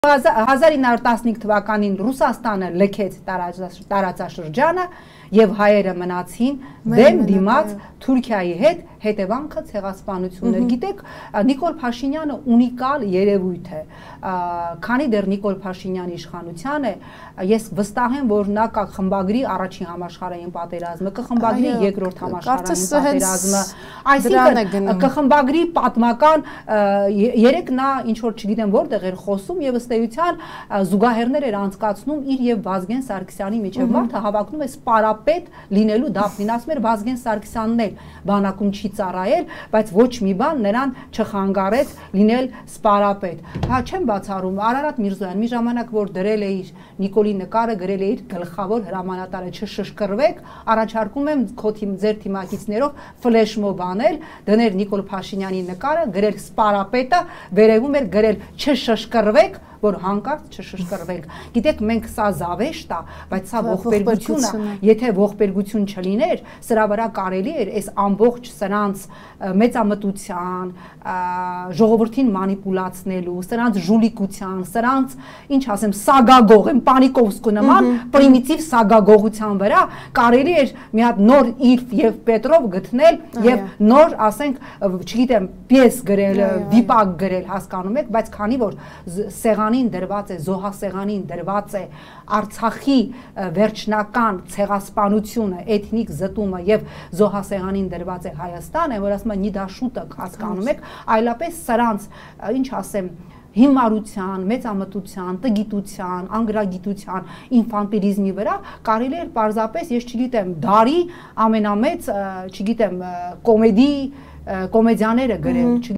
Դրեք Փաշինյանի նկարը ու գրեք՝ սպարապետը, չշշկռվեք․ սա զավեշտ է, եթե այսքան ողբերգություն չլիներ
Հյուրը «Անվտանգություն և ժողովրդավարություն» ՀԿ նախագահ, ԱԺ նախկին պատգամավոր Նաիրա Զոհրաբյանն է։
Հարցազրույցն ամբողջությամբ դիտեք այստեղ։